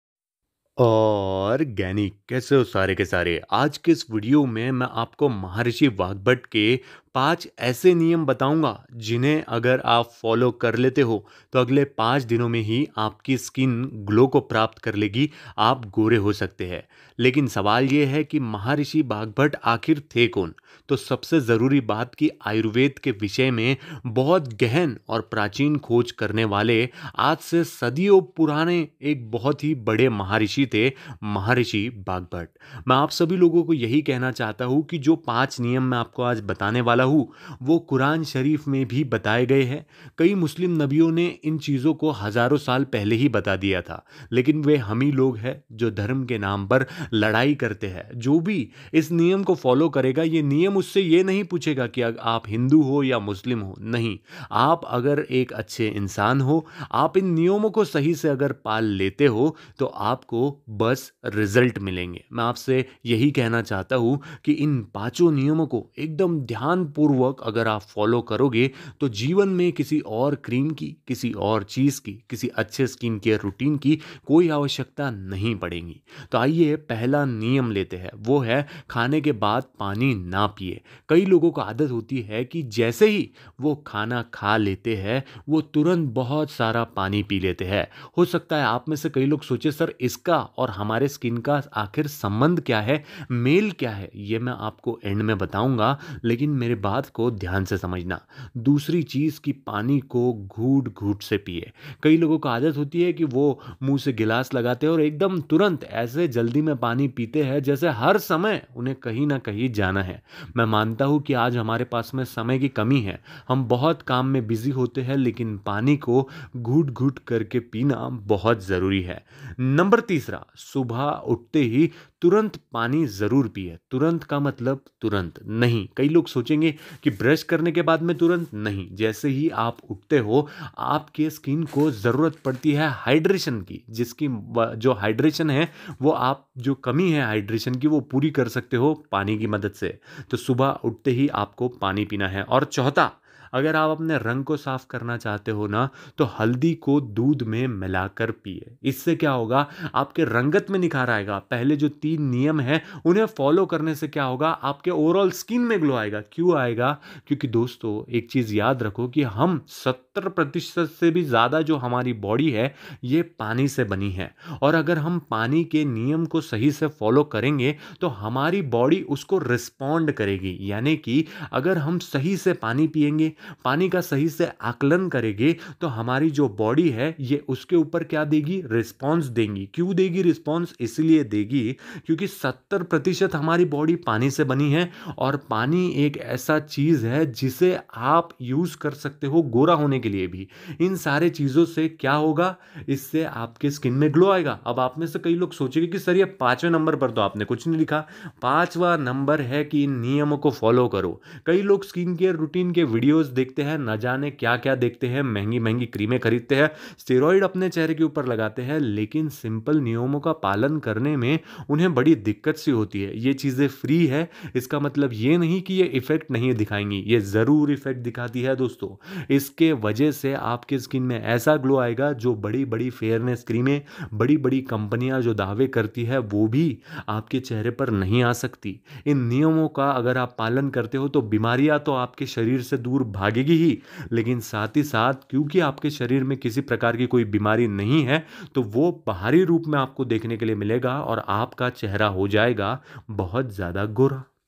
महारिशी वागभट 👉,आप 5 दिन में गोरे होंगे बस पानी ऐसे पीना,
आर्गेनिक कैसे हो सारे के सारे आज के इस वीडियो में हमने बात की है कुछ ऐसे सूत्र की जीने आज से सदियों पहले एक महान आयुर्वेदिक ऋषि महारिशी वागभट जी ने बताया था तो आज का ये वीडियो आप पूरा जरूर देखो जिसका ये कहना है की अगले 5 दिन आप पानी को इस तरीके से पियो तो शरीर में होने वाले रोचक बदलाव और चेहरे पर आने वाले अनोखी चमक को आप देखते ही रहे जाओगे अगले 5 दिन सिर्फ 5 दिन आपको ये सूत्र अपना कर इसे अपने जीवन का एक भाग बनाना है